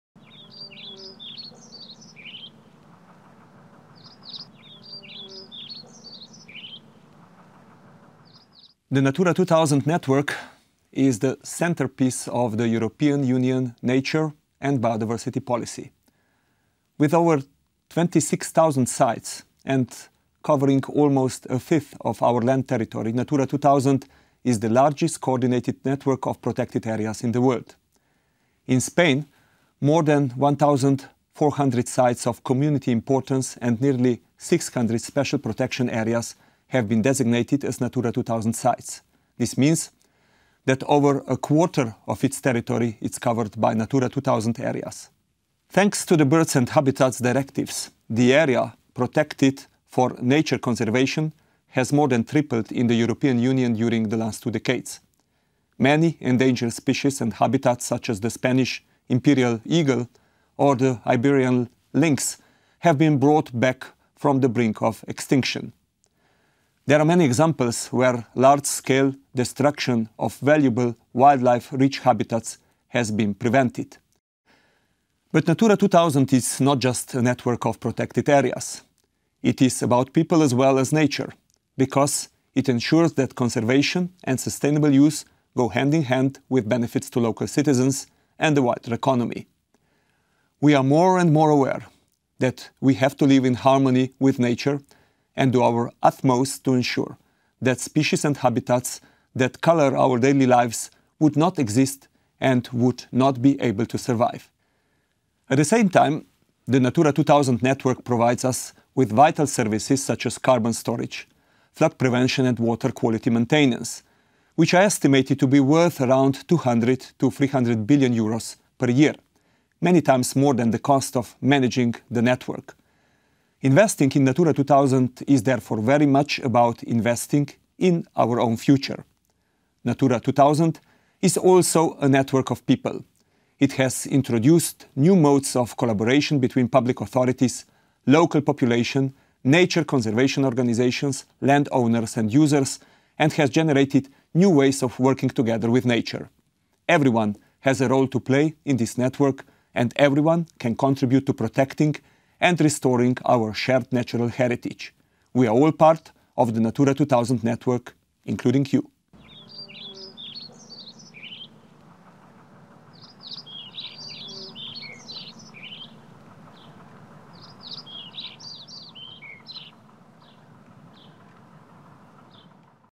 Janez Potocnik
Tras cinco años de mandato como Comisario Europeo de Medio Ambiente,  Janez Potocnik se prepara para dejar su cargo con una clara apuesta por la Red Natura 2000 como pieza clave para la conservación de la biodiversidad europea.